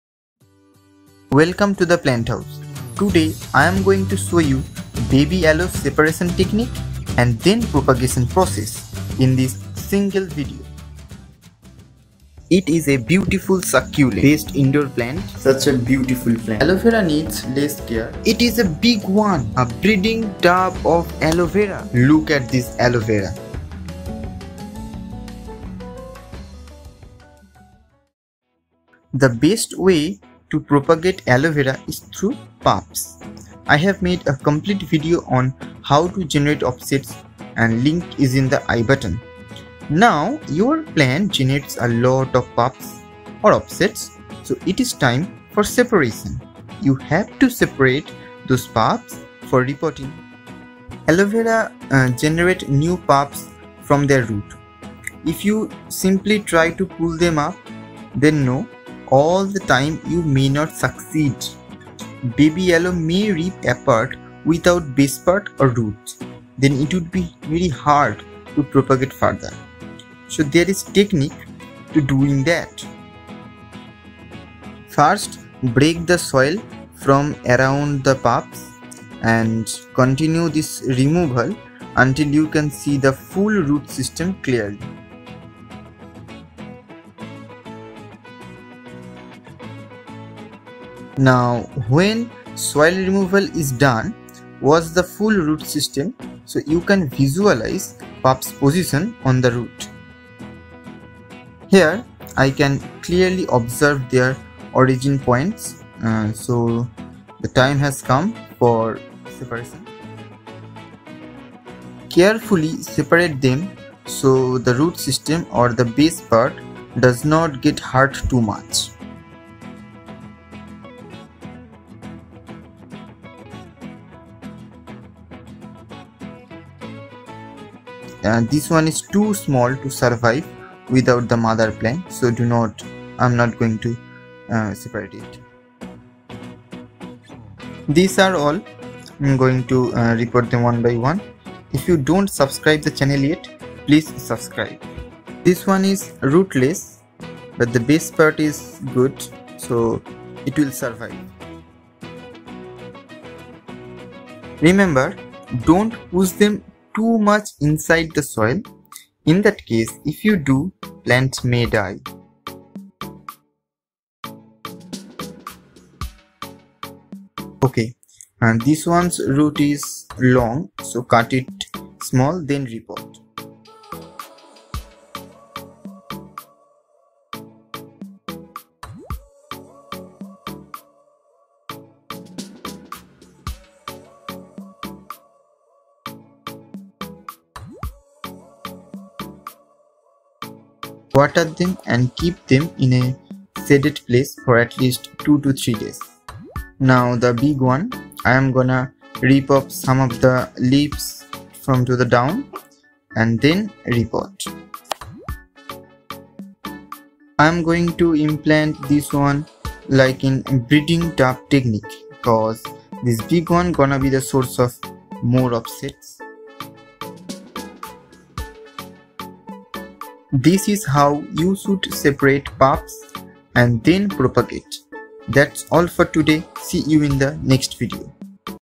Aloe Vera offsets/ pups separation \/ separation to propagation technique of Aloe Vera \/ aloe vera
Welcome to the plant house.
 In this single video I've shown baby aloe pups / offsets separation technique and then propagation process.
Go through the full video and comment.

For Quick watch -

0:00 - start
0:29 - separation process
2:18 - propagation process
3:16 - mother aloe